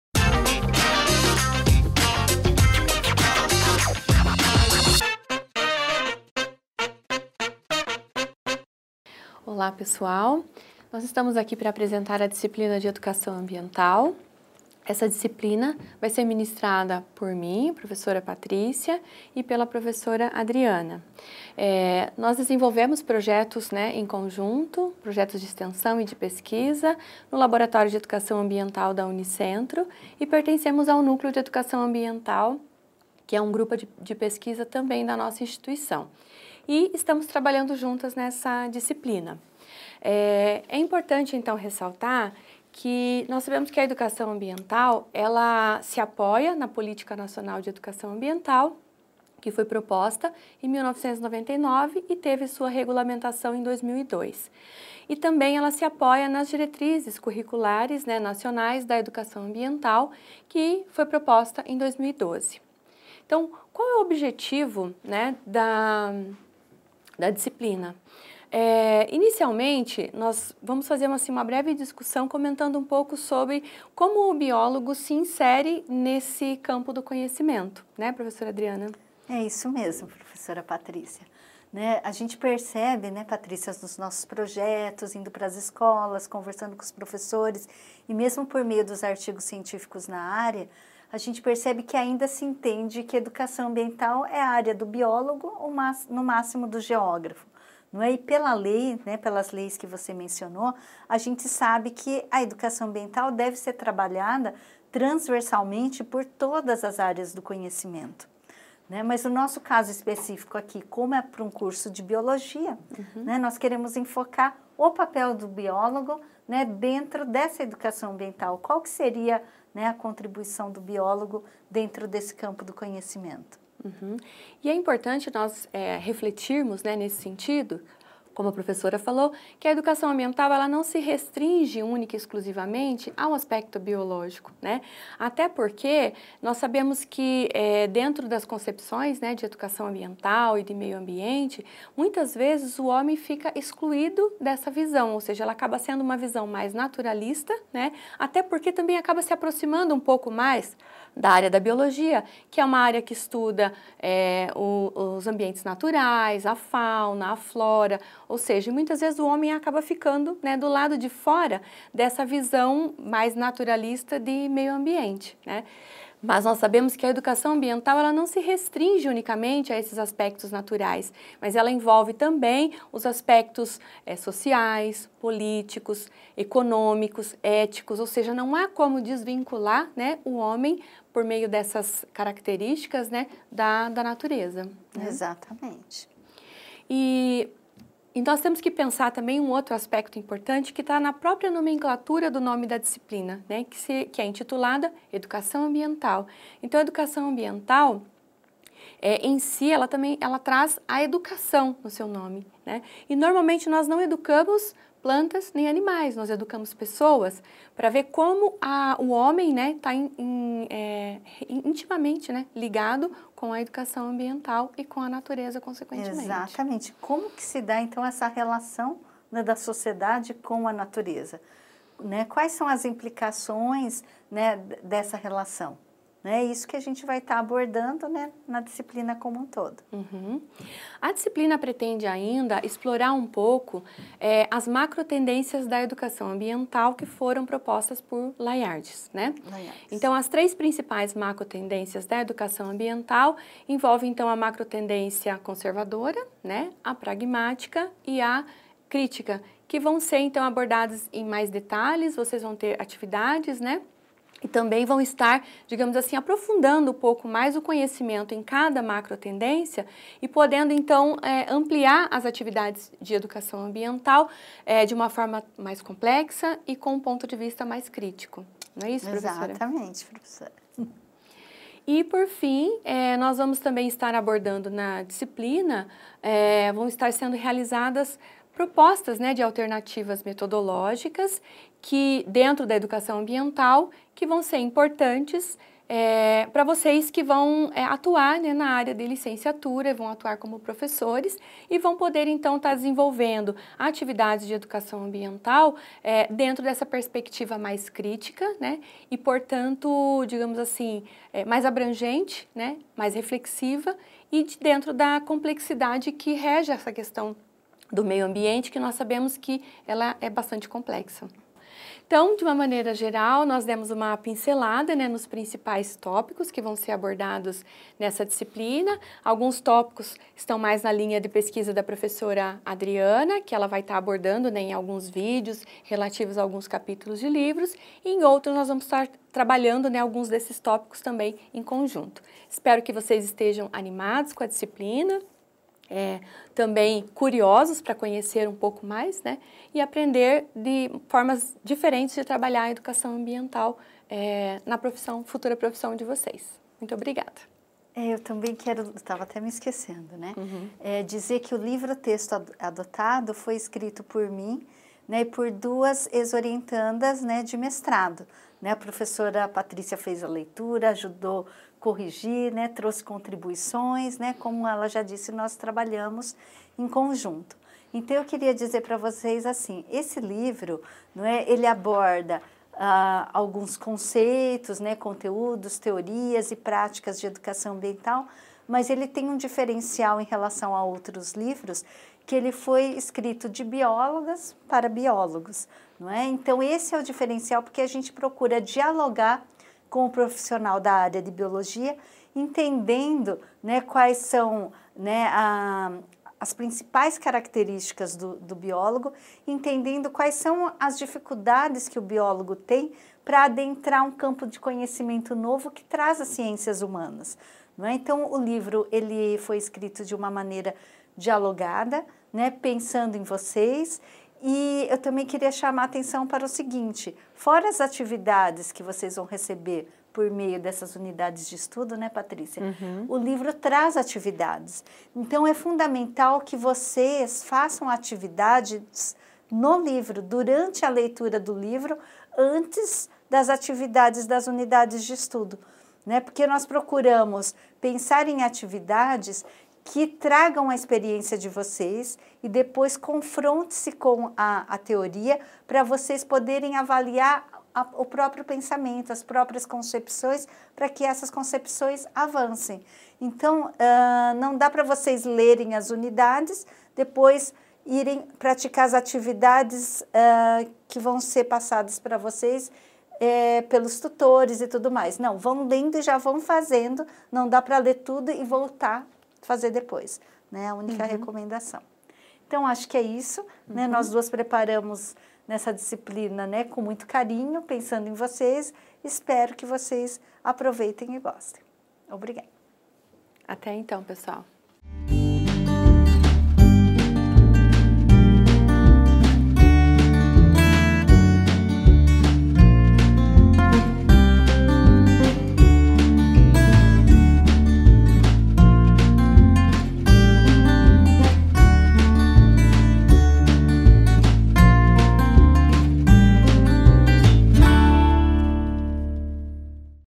[BIO] Educação Ambiental - Apresentação
Tema Educação ambiental para o curso de Biologia com as professora Patricia Giloni e Adriana Kataoka.  Apresentação